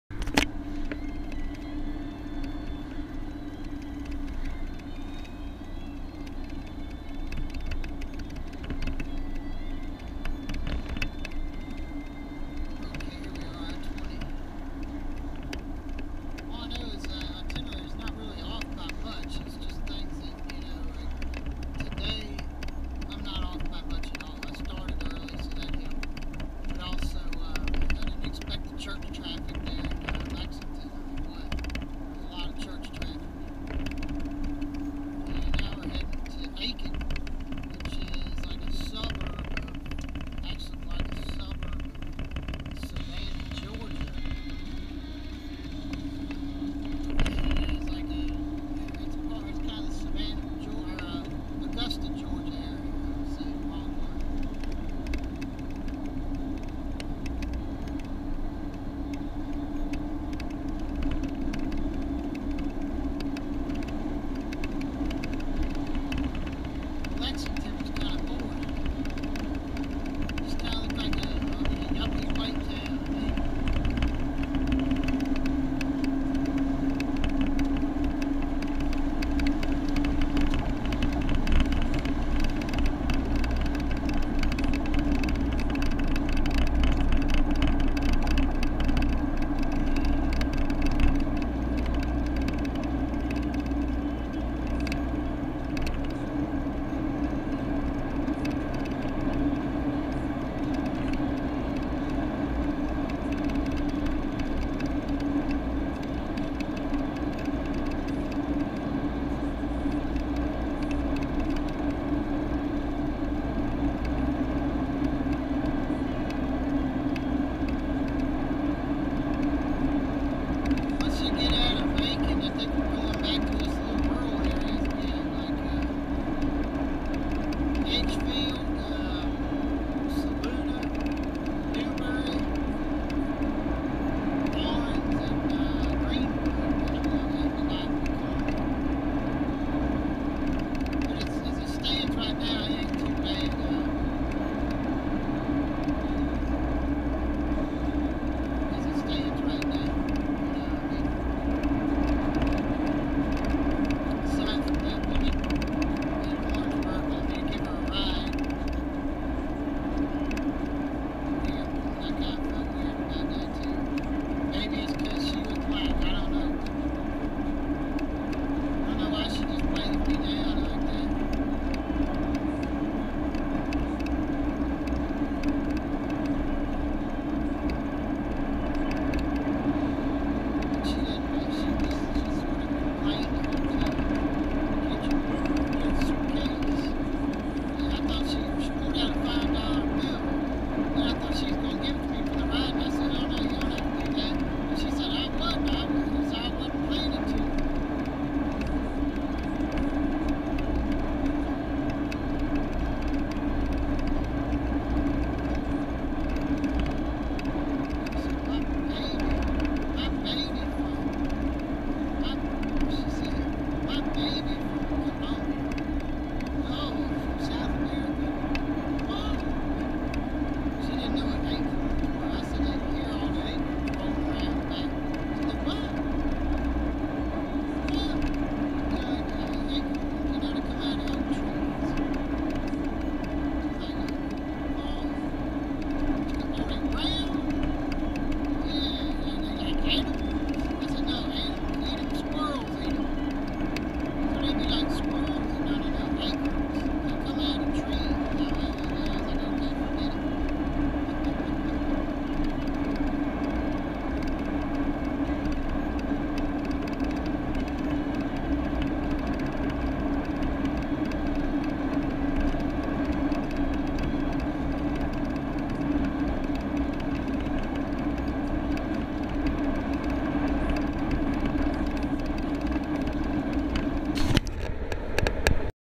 I-20 from Lexington SC toward Aiken SC
US Route 1 onto I-20 west,January 18,2015